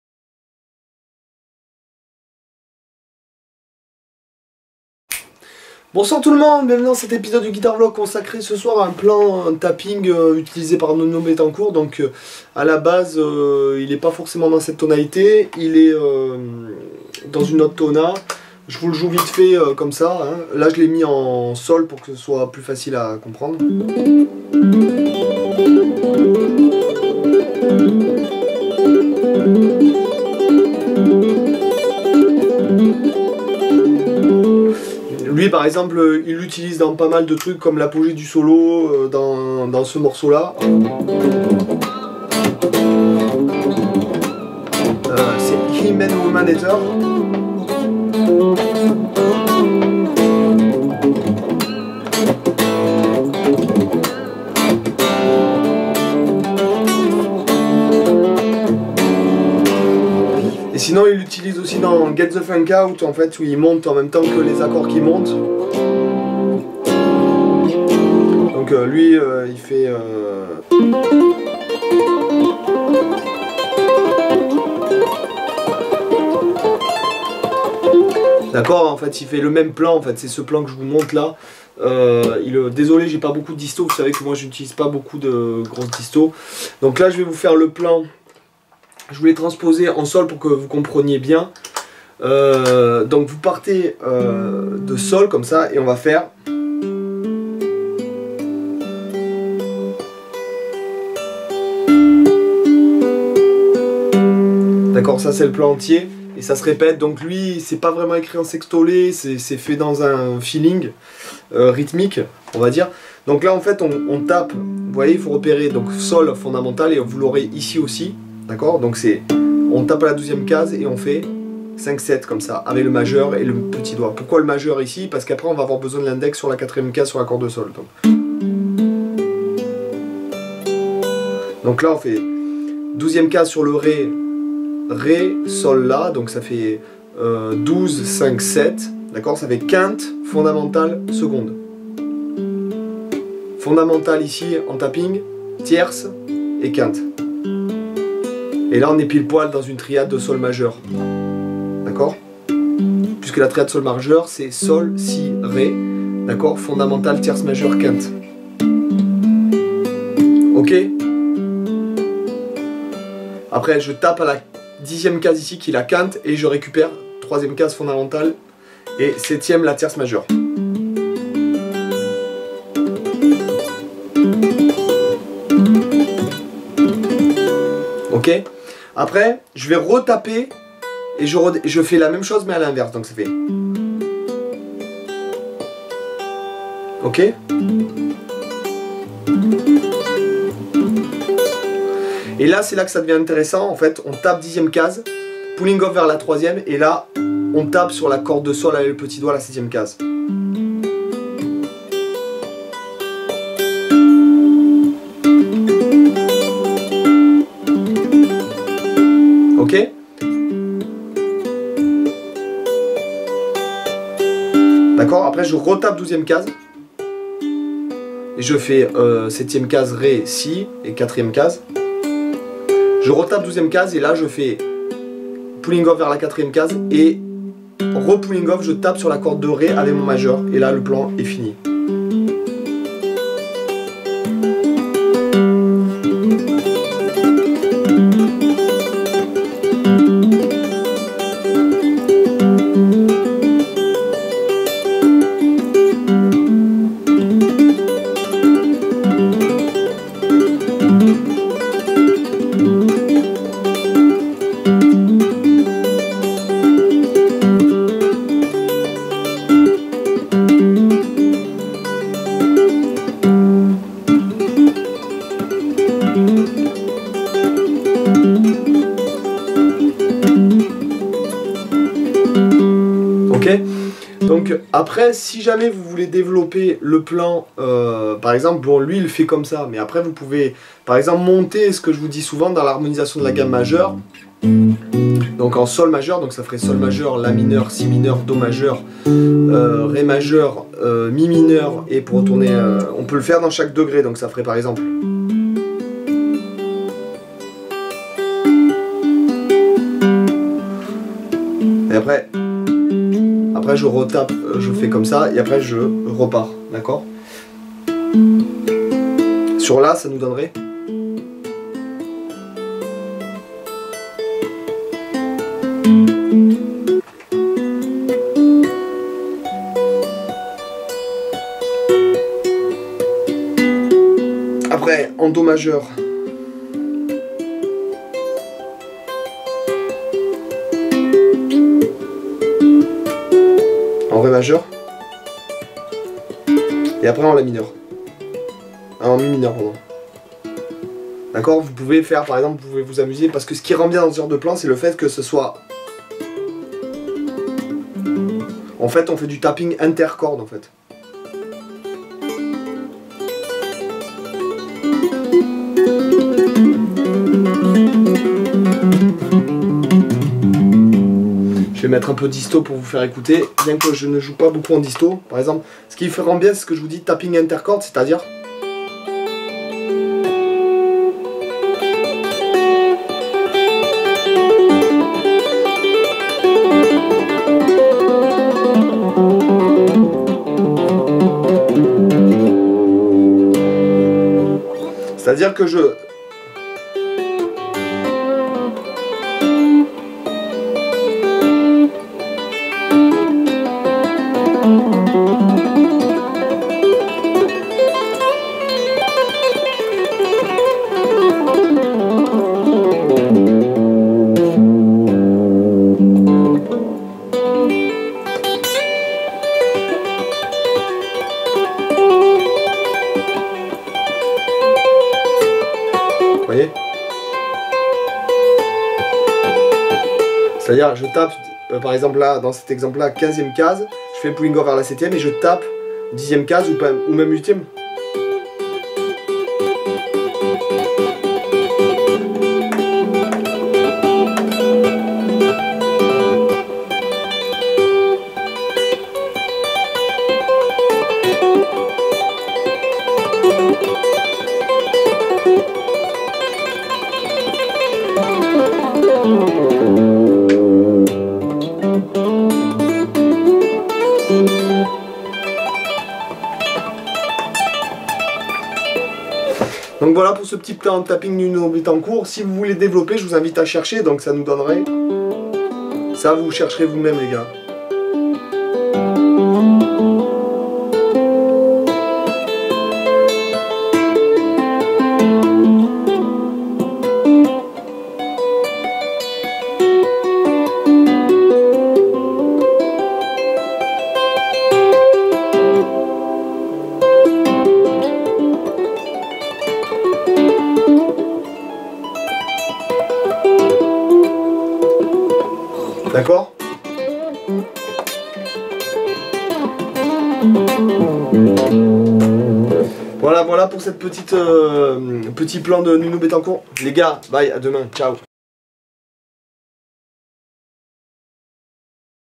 NUNO BETTENCOURT TAPPING LICK - LE GUITAR VLOG 318
voilà une petite phrase en tapping de nuno bettencourt